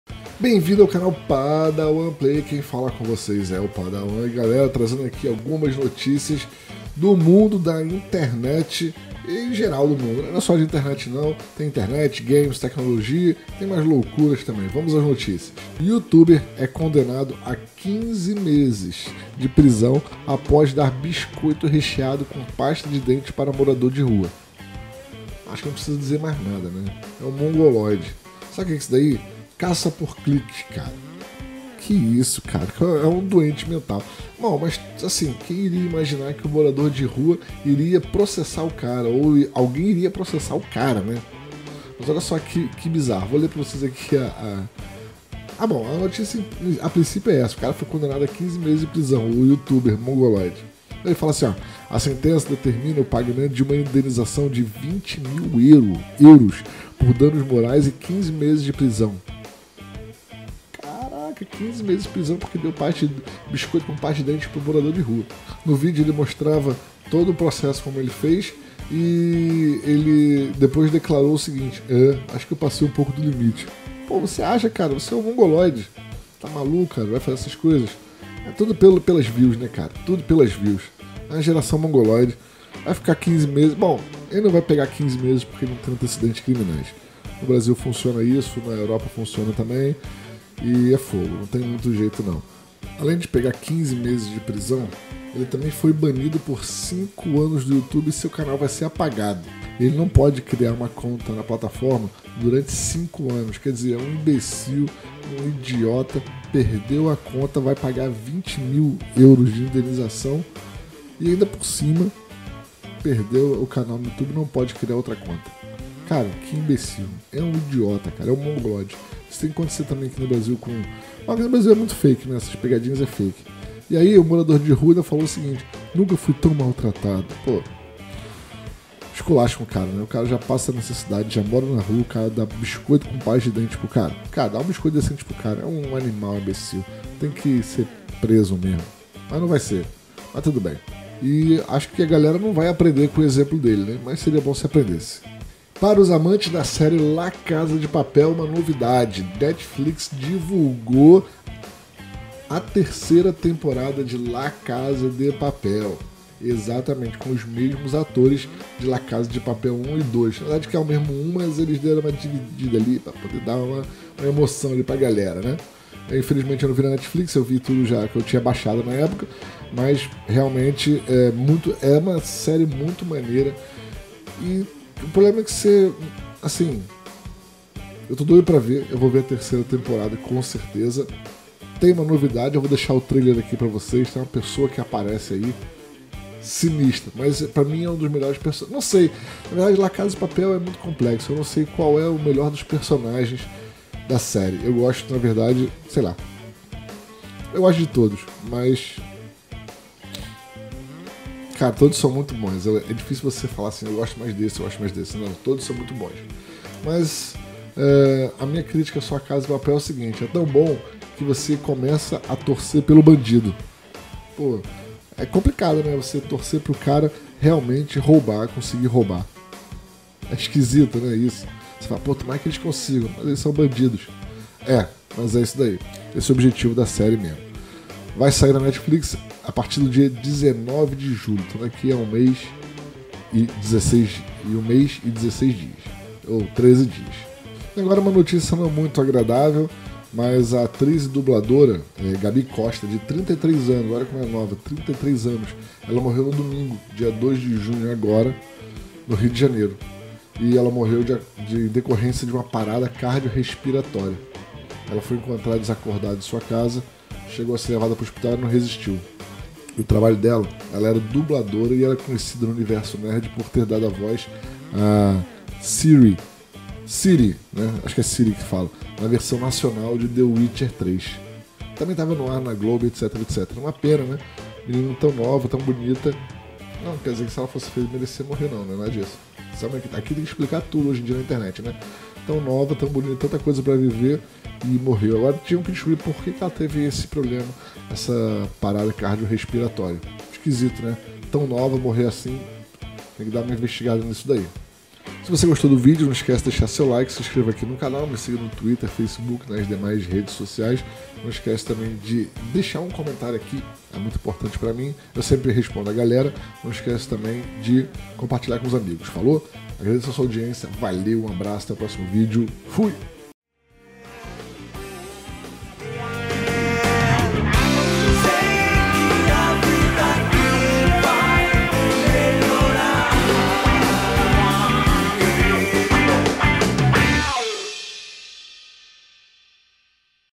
LA CASA DE PAPEL 3, YOUTUBER PRESO, DUBLADORA DE THE WITCHER
Video com as noticias sobre o youtuber preso, La Casa de Papel 3, e sobre a morte da dubladora de the witcher.